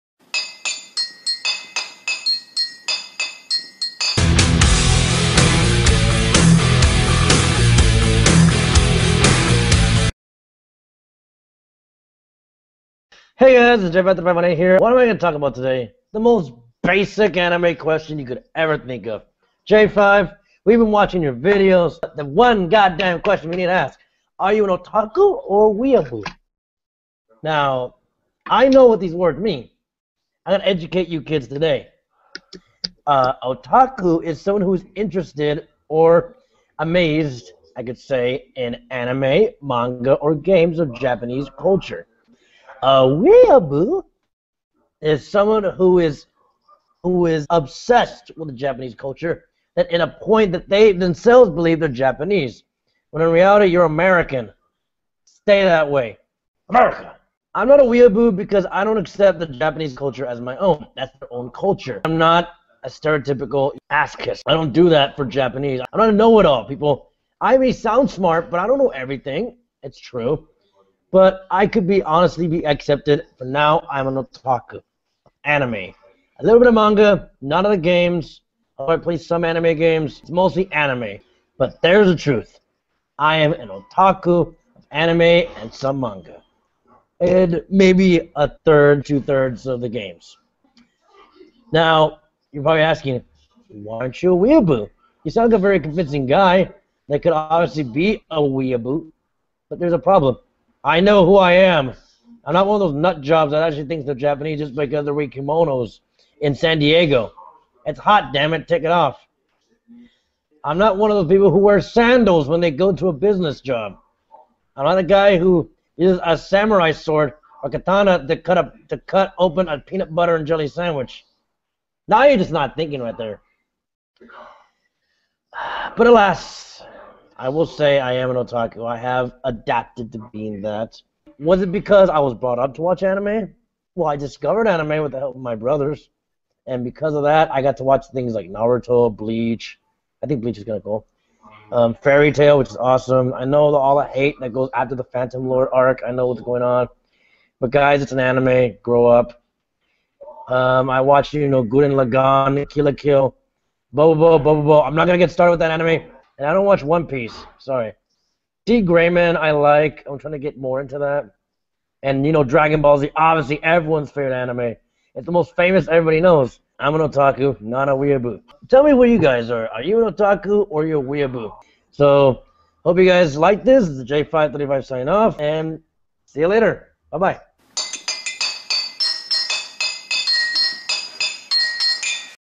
Otaku or Weeaboo - J53518
J53518 educates you on what an otaku or a weeaboo means as well as telling you whether he is an otaku or a weeaboo for this week's episode of Anime Chat Mondays with J53518.